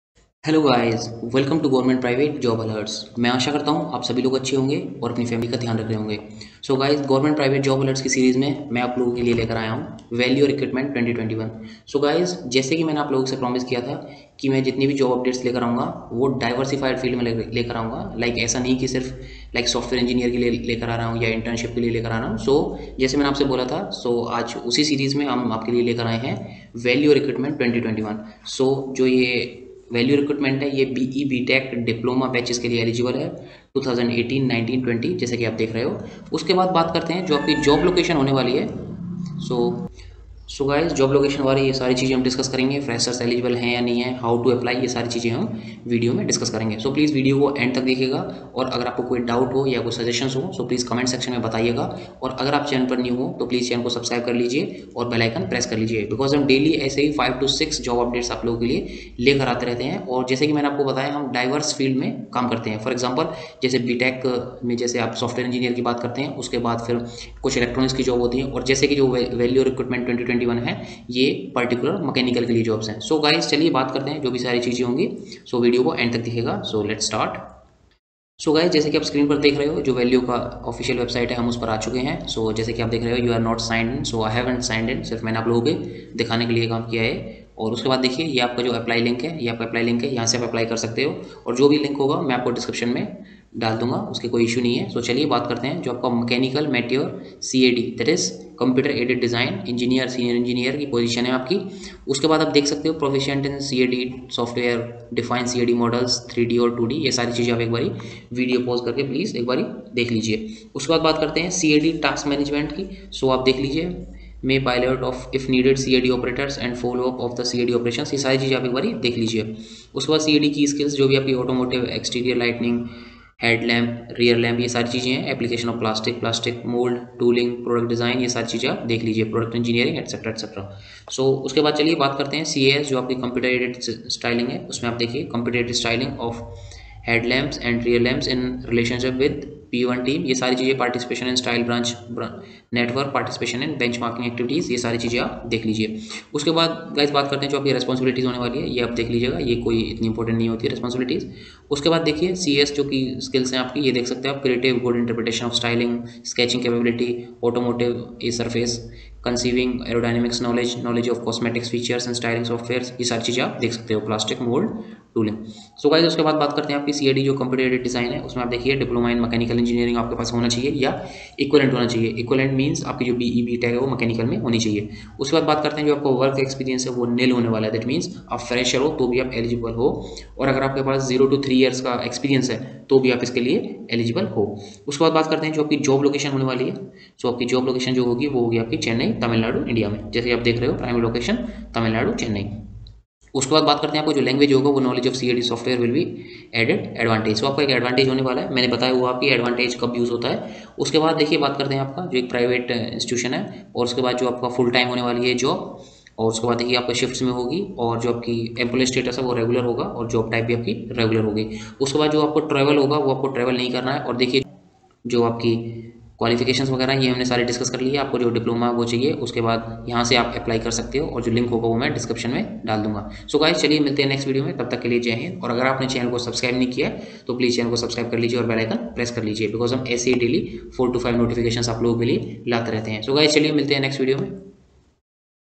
Valeo Recruitment 2021||Freshers||B.E/B.Tech/Diploma||Eligibility||Batch 2018|2019|2020||Job Loc
In this video we have discussed aboit the job opportunity at Valeo Recruitment 2021||The job opening is for B.E/B.Tech/Diploma of Mechanical Branch||
Please watch the complete video to get full information of the Job recruitment||